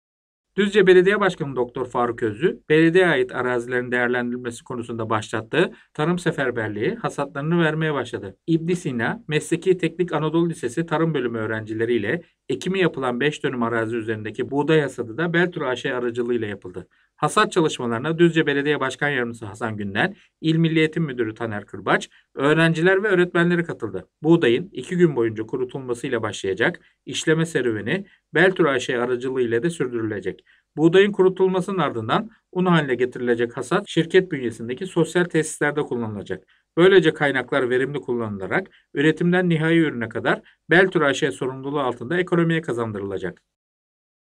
Buğday Hasadı Yapıldı
Düzce Belediye Başkanı Dr. Faruk Özlü’nün, Düzce’de tarım lisesinde öğrenim gören öğrenciler ile birlikte ekim yaptığı buğdayın hasadı yapıldı. 5 dönüm arazi üzerinde yapılan hasattan elde edilen buğday Beltur A.Ş. tarafından işlenerek tesislerde kullanılacak.